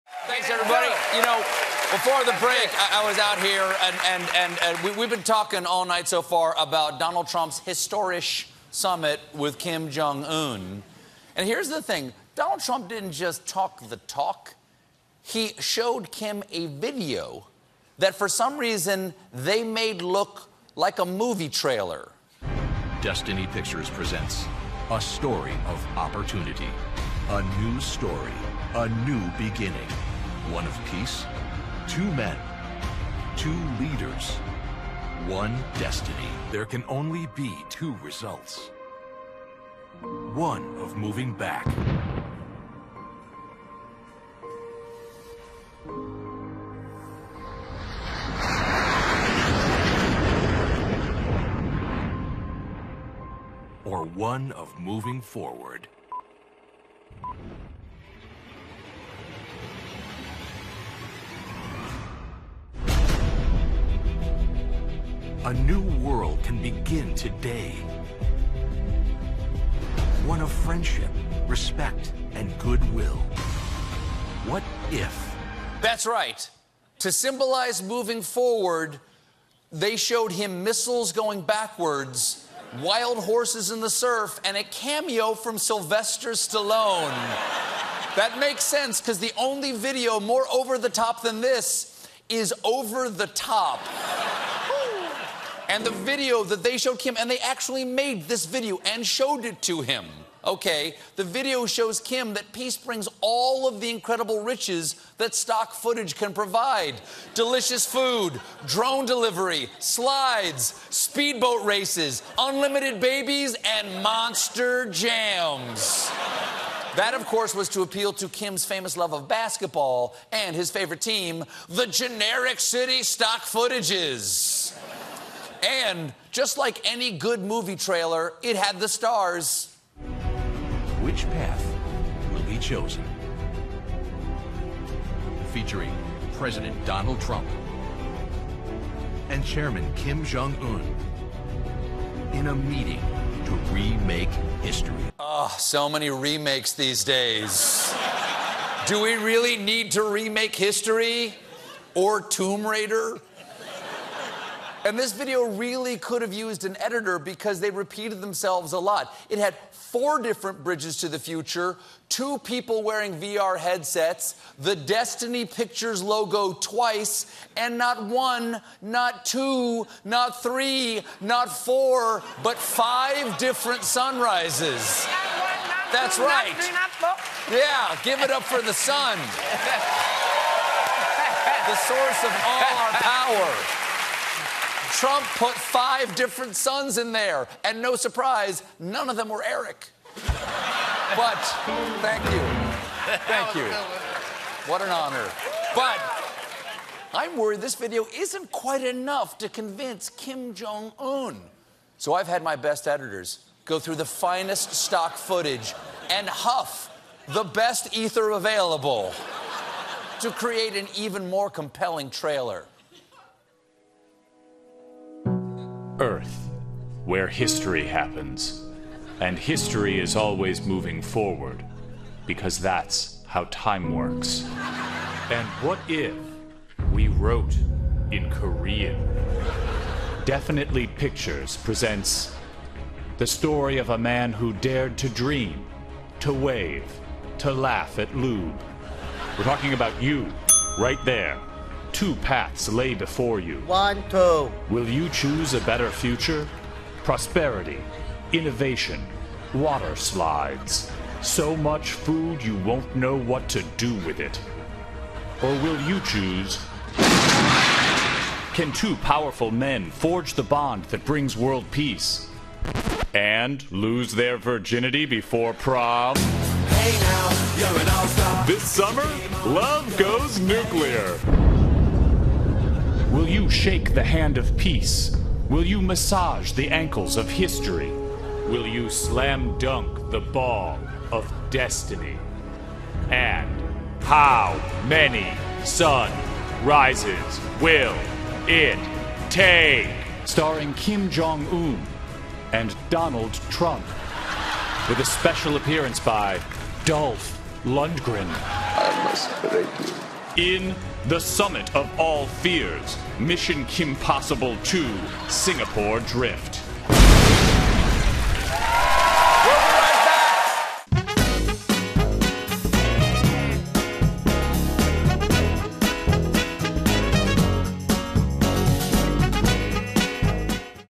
The Odd Movie Trailer Trump Showed Kim Jong Un
A strange movie trailer the White House made for Donald Trump to show Kim Jong-un was filled with dramatic voiceover, stock footage, and Sylvester Stallone.